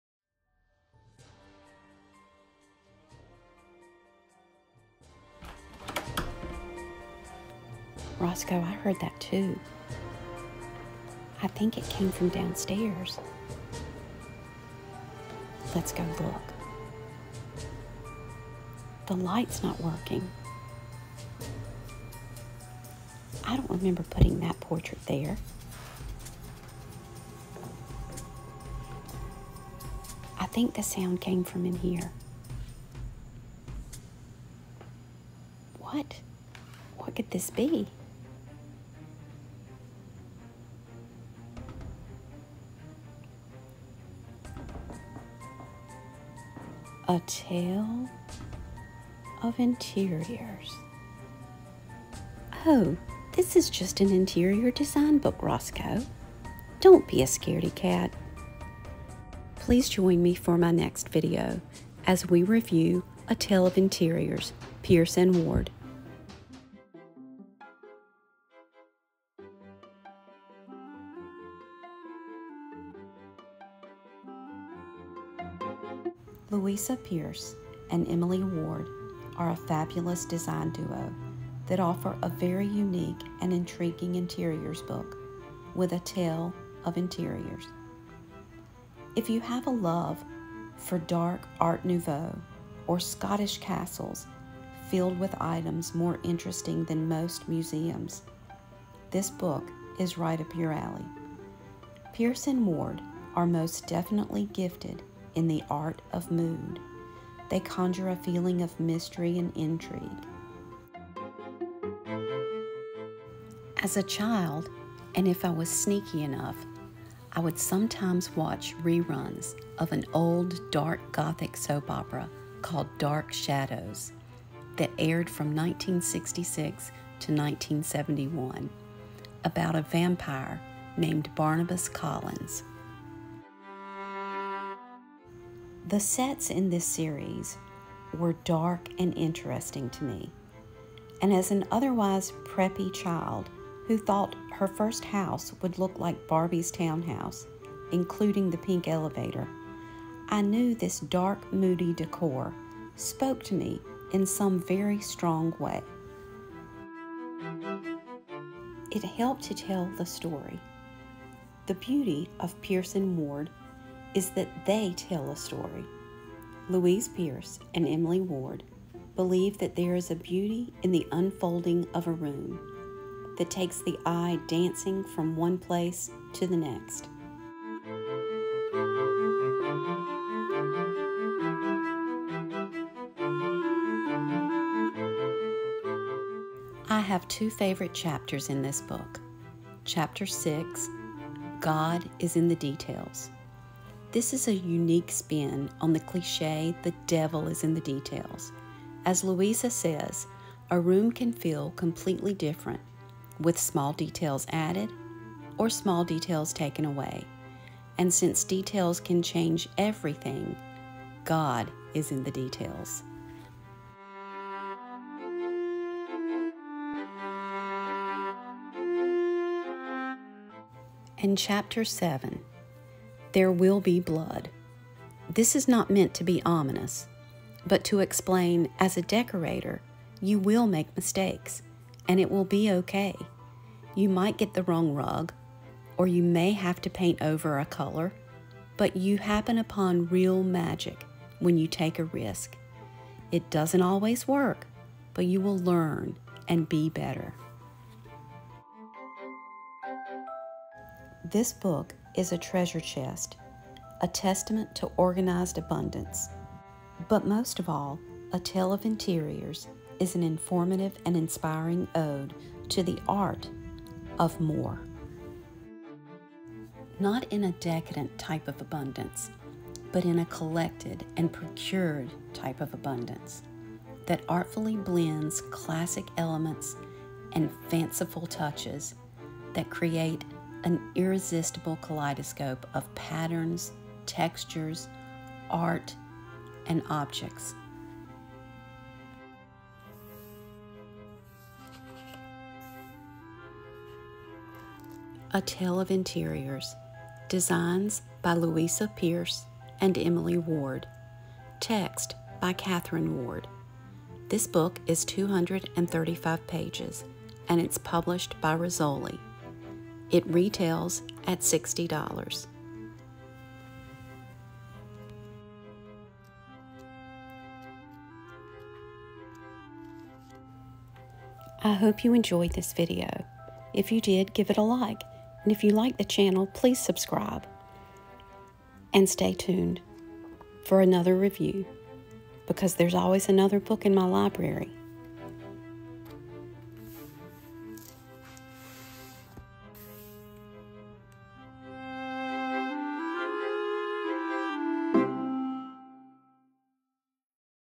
A Review of: A Tale of Interiors by Louisa Pierce & Emily Ward Designs - Text by Catherine Ward
Louisa Pierce and Emily Ward are a fabulous design duo that offer a very unique and intriguing interiors book with A Tale of Interiors
If you have a love for dark art nouveau or Scottish castles filled the items more interesting than most museums this book is right up your alley.
Pierce and Ward are most definitely gifted in the art of mood. They conjure a feeling of mystery and intrigue. The beauty of a Pierce & Ward home is that they tell a story. Louisa Pierce and Emily Ward believe that there is a beauty in the unfolding of a room that takes the eye dancing from one piece to the next. This book teaches readers about organized abundance with un-gaudy decadence, it's an inspiring ode to the art of more.